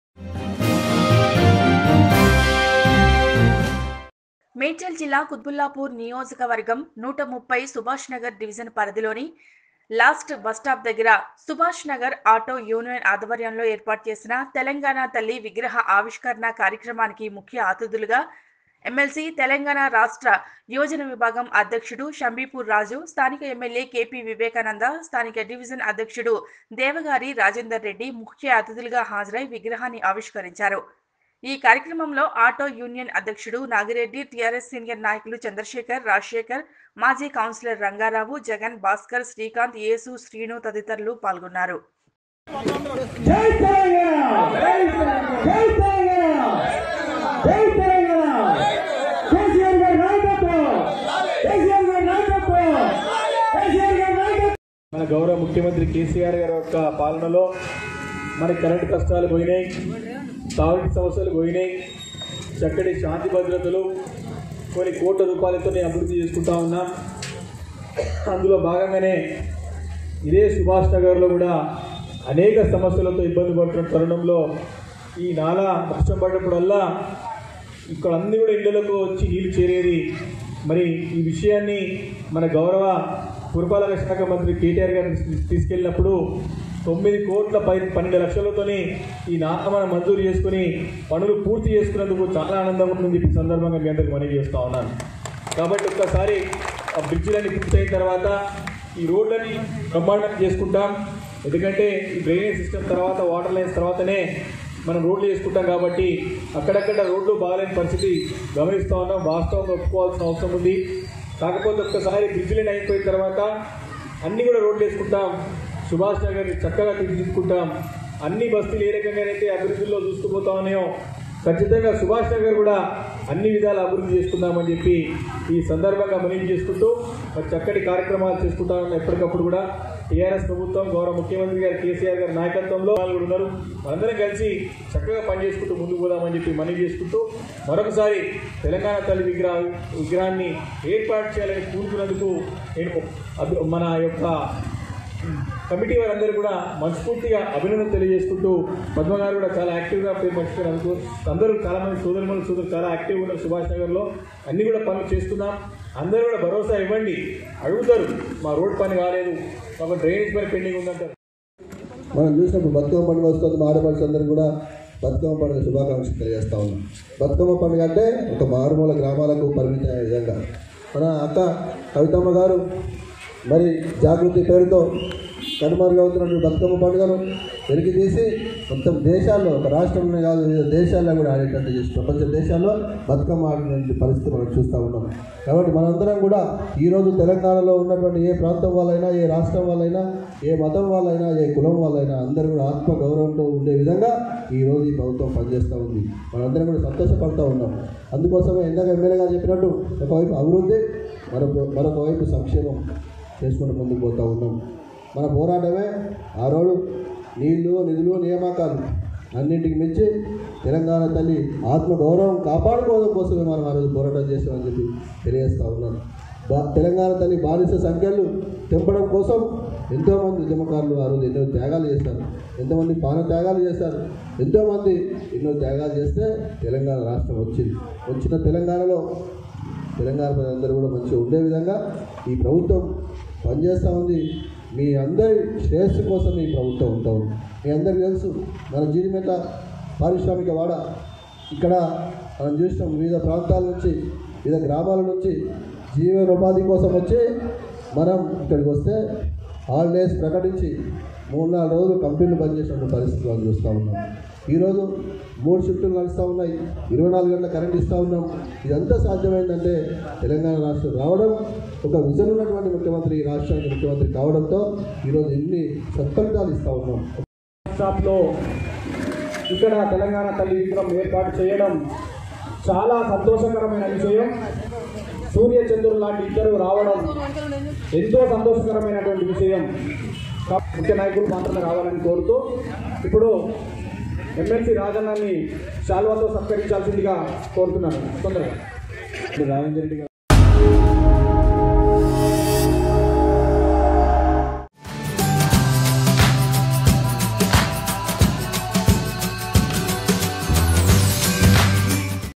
సుభాష్ నగర్ ఆటో యూనియన్ ఆధ్వర్యంలో తెలంగాణ తల్లి విగ్రహ ఆవిష్కరణ || Nethra News
|| Nethra News || Subash Nagar ||